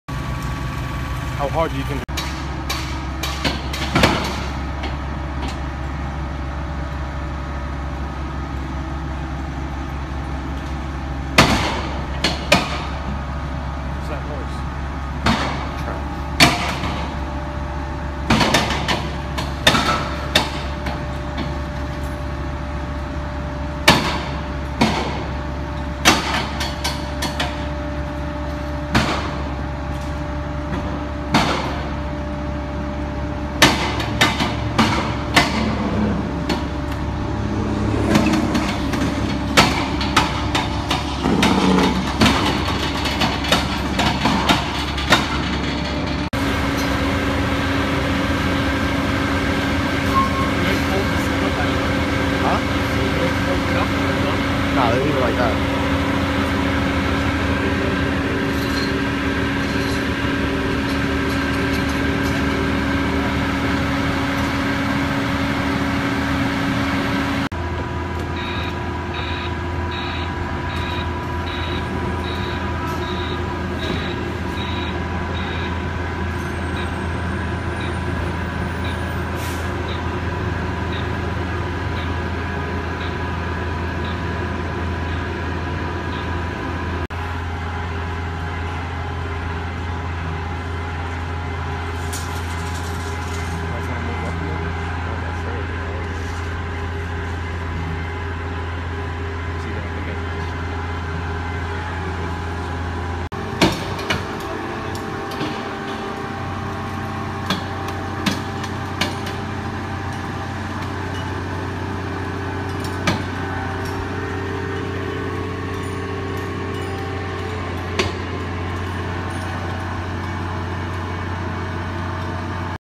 Liebherr LR1400 working
Amquip's Liebherr LR1400 crawler crane crawling, setting a huge concrete section down, booming down and ending the day at the Bayonne Bridge project in Staten Island NY.